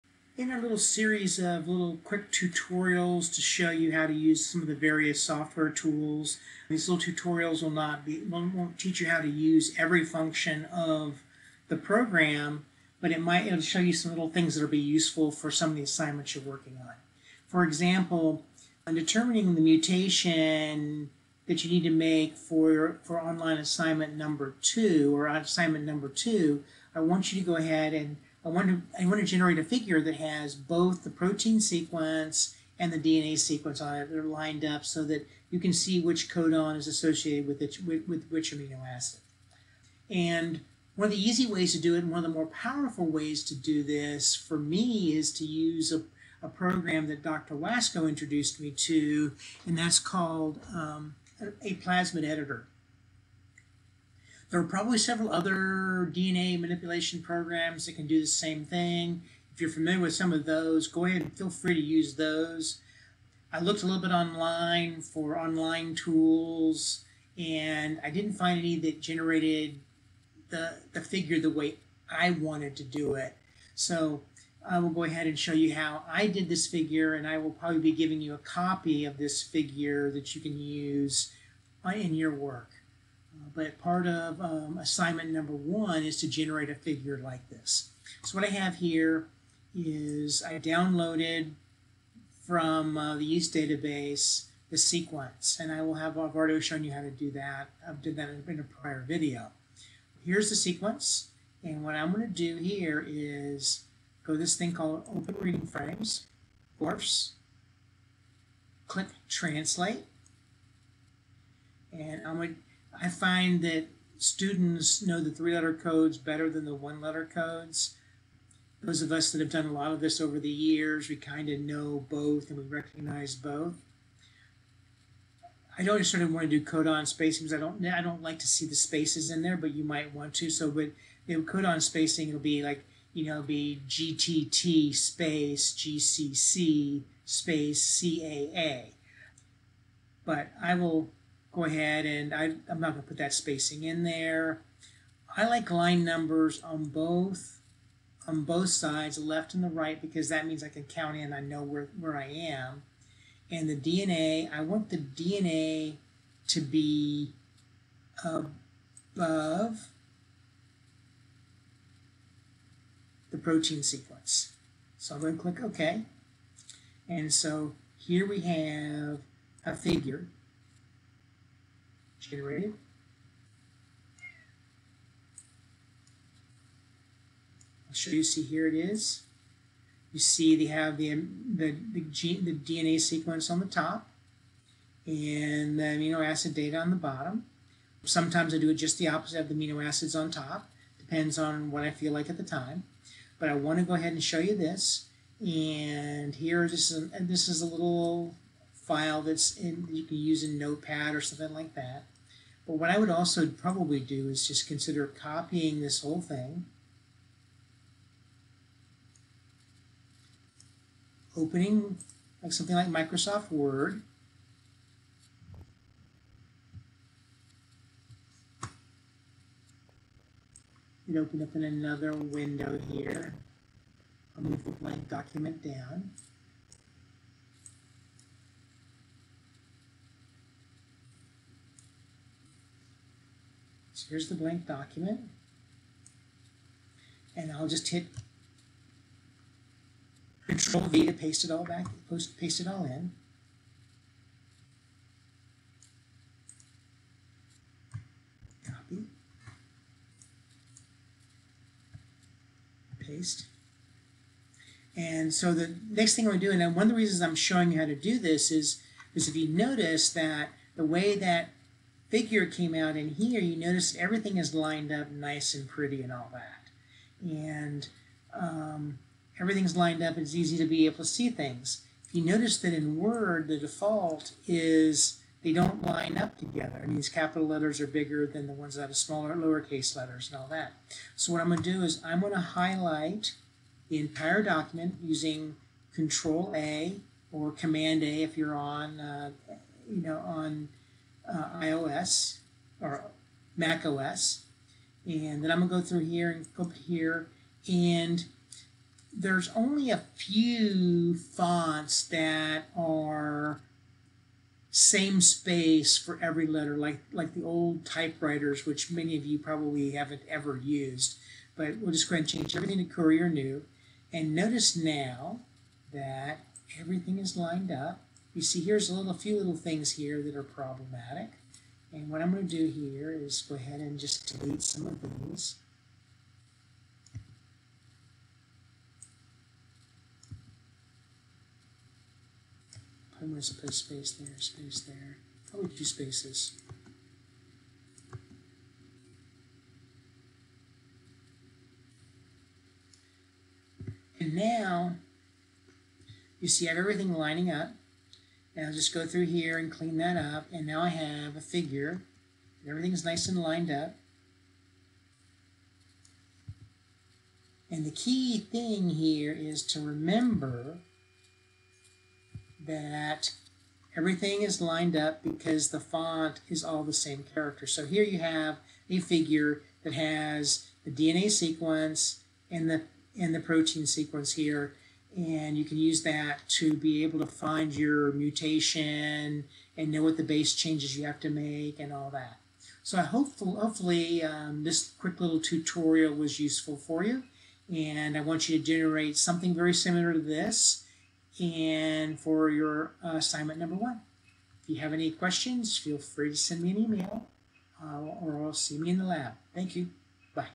Using ApE to Create a DNA and Amino Acid Sequence Figure
I will walk student through using ApE (A plasmid Editor) to align DNA and protein sequence and create a figure.